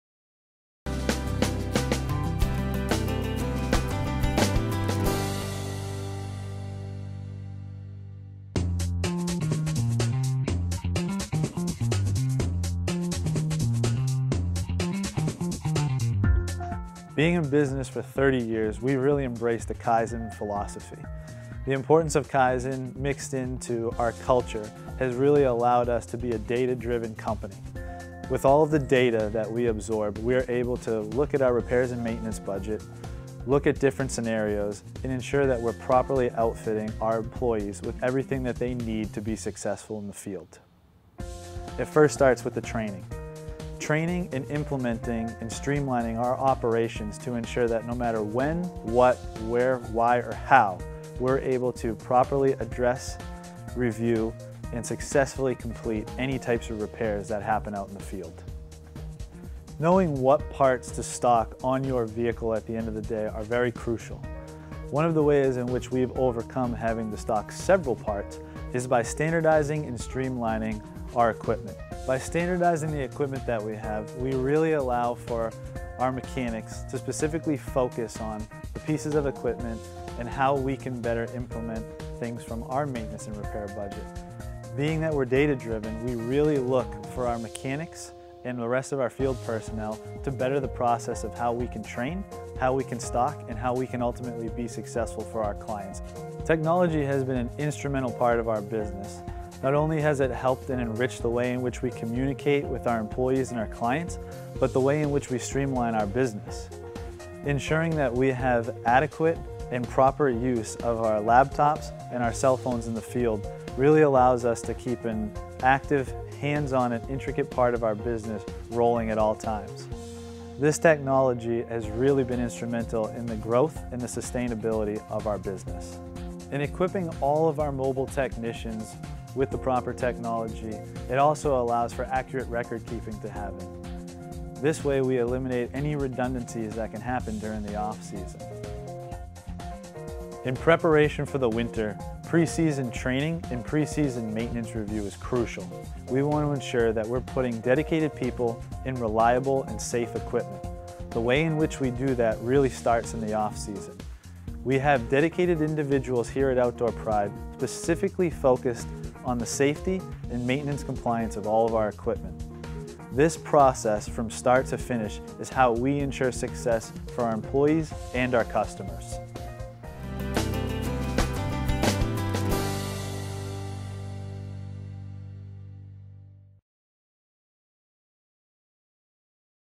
Snow Maintenance Processes and Training
Outdoor Pride Landscaping of Manchester, NH discusses how data is used in every aspect of their snow and ice management company, including training and maintenance, stocking equipment, and using technology for accurate record keeping.